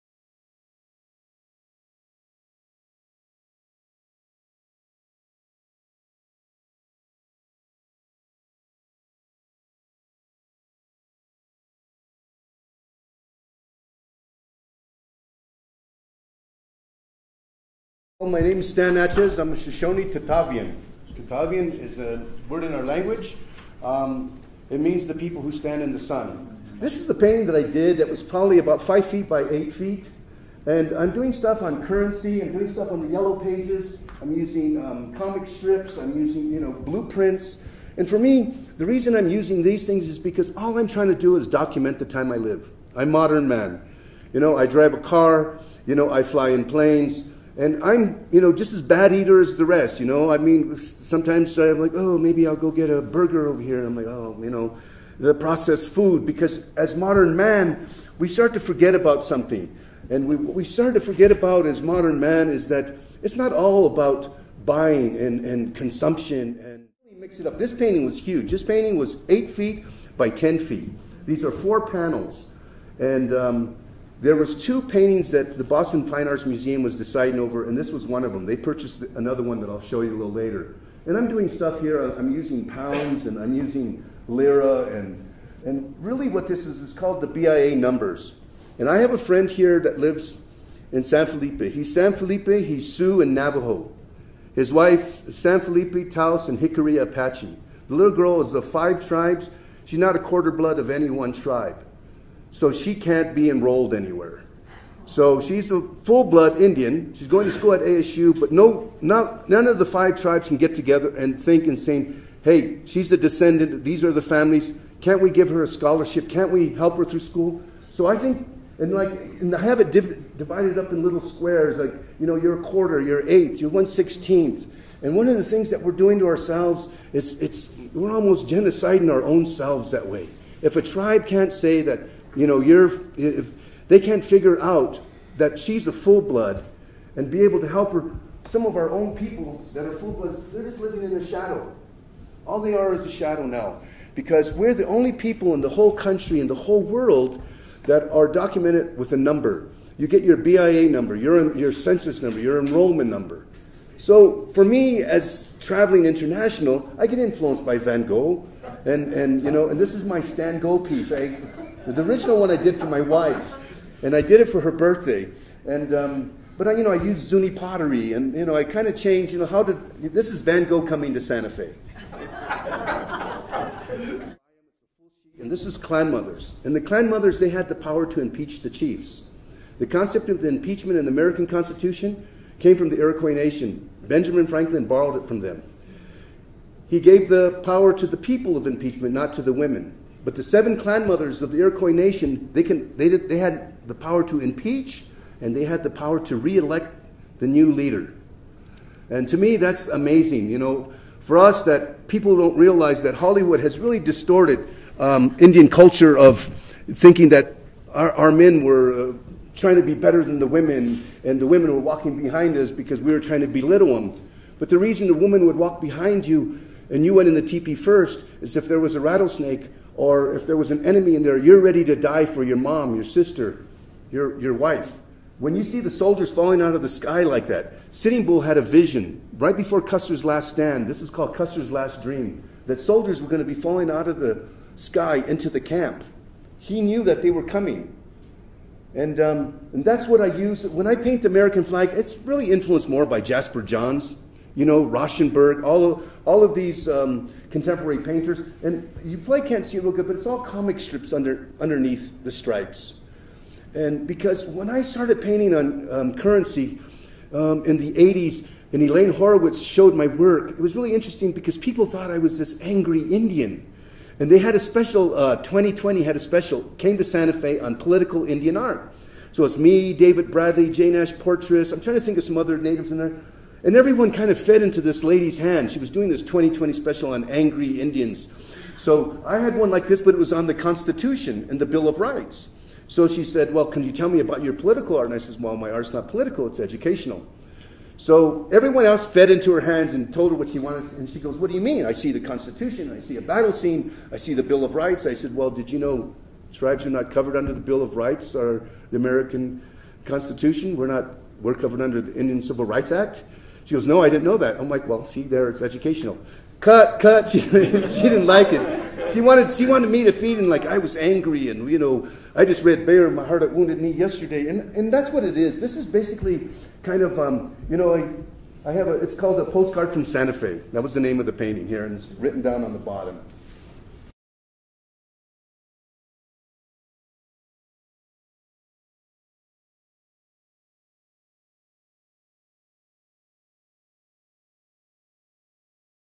Highlights of Shoshone painter Stan Natchez at MIAC
Shoshone painter Stan Natchez talks about his life and his work in a presentation at the Museum of Indian Arts and Culture in Santa Fe. These highlights serve as an introduction to the full presentation available at Youtube.com/IndianArtsCulture.